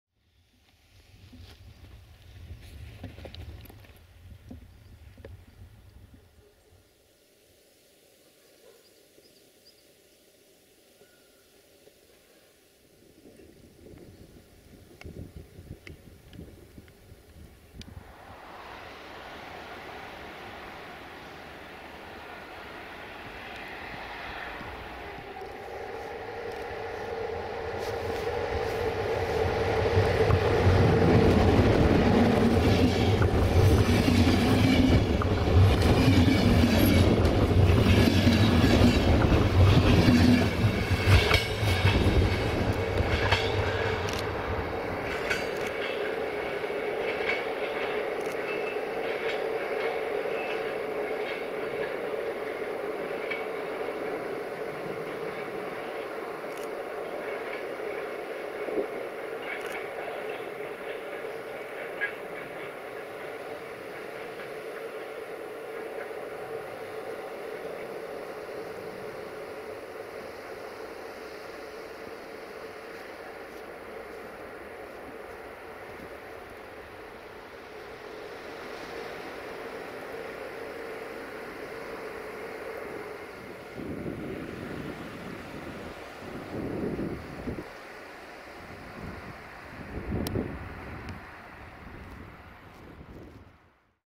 Delfinul 477-551-2 & IR1634 Brasov-Bucuresti Nord in Zapada/Snow in Predeal - 02 March 2018
1:IR1634 - Brasov - Bucuresti Nord - Delfiul~060-EA/LE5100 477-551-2 RO-SNTFC

02 March 2018